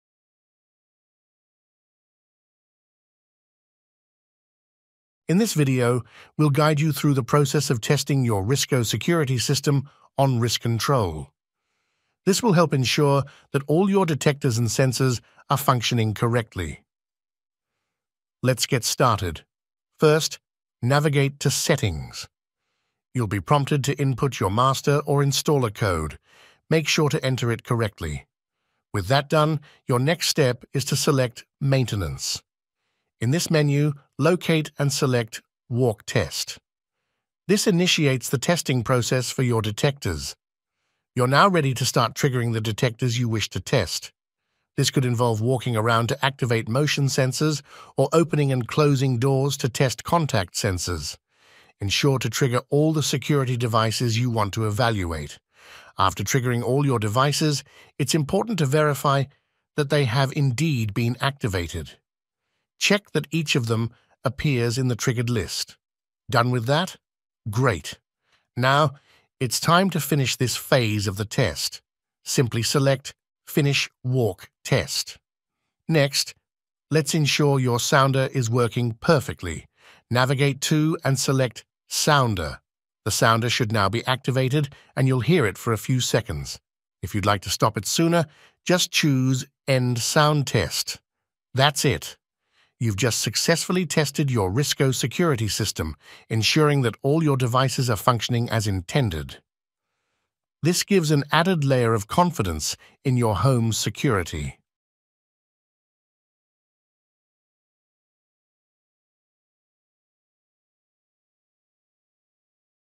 How to Test your RISCO Security System on RISControl
To make sure that all your detectors and scenes are working correctly, you need to test your RISCO security system. In this video, you will be guided through the process of testing your system.